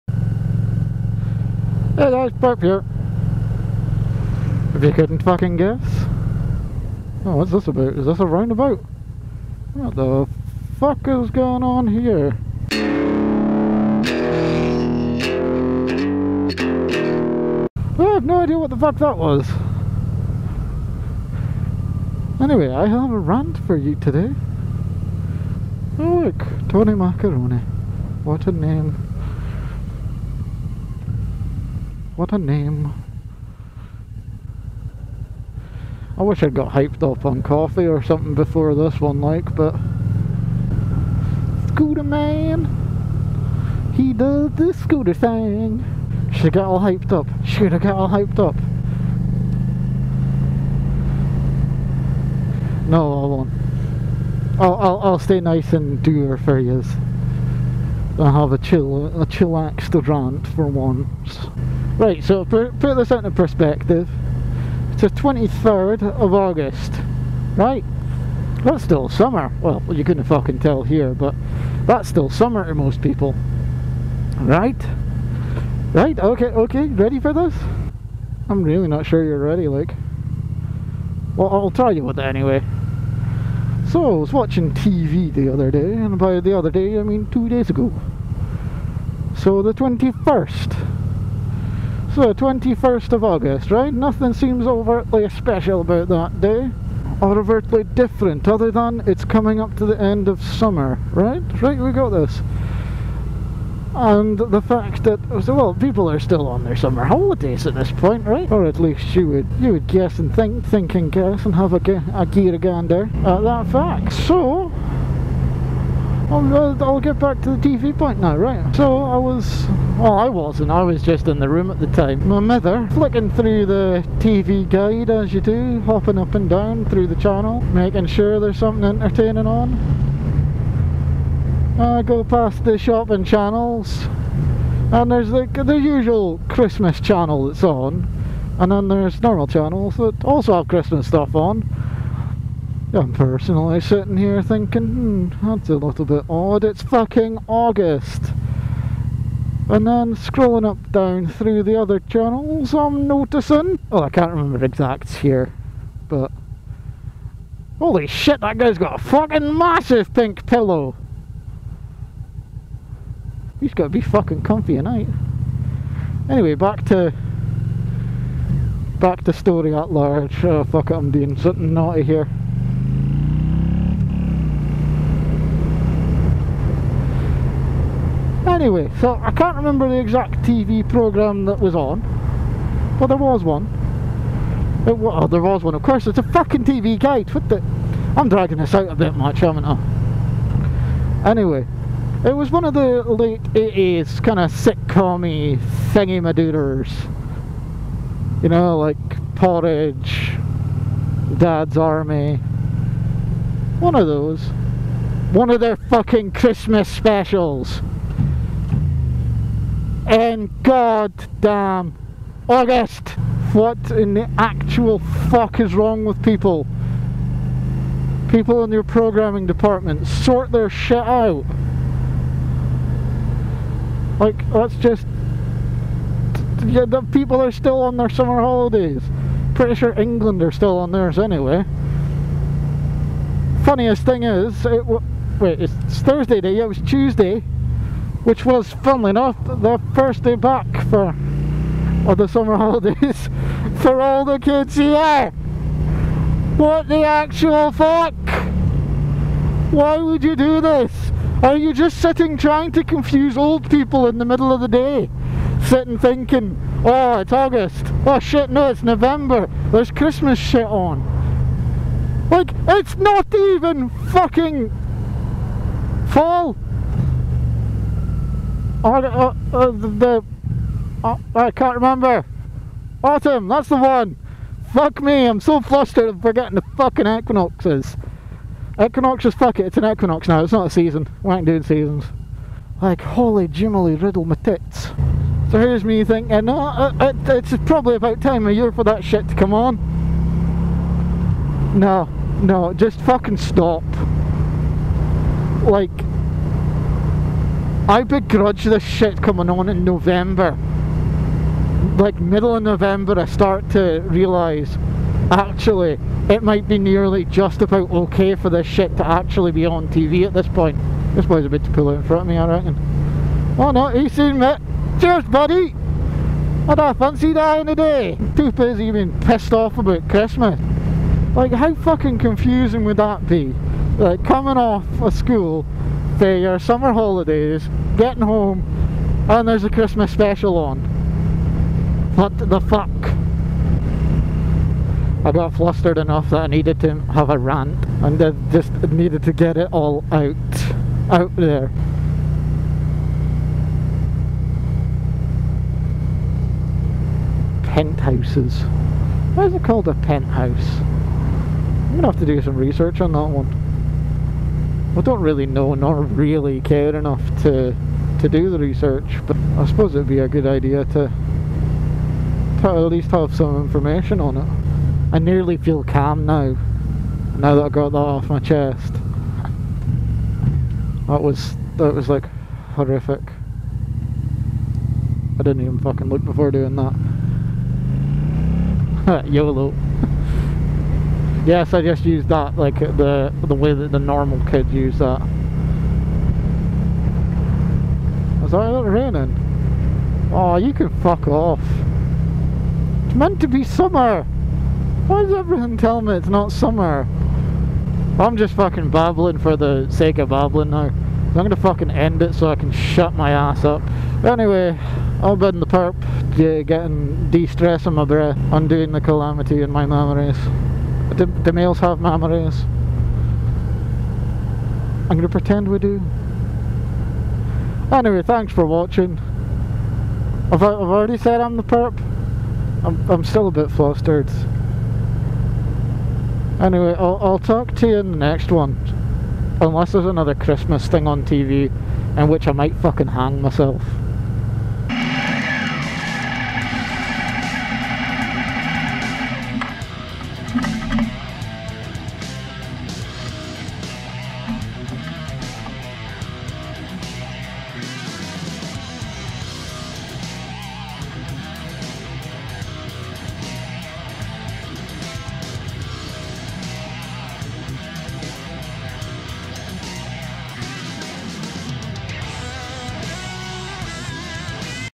Bloody TV | Rant | Suzuki SFV650 | Scottish Motovlog
i had a moan, it was good.

EQUIPMENT

Camera :Sena C10 (mic and all that jazz is integrated)
Bike : Glados (suzuki sfv 650/Gladius)

Anything else? just ask! smash a coment below asking.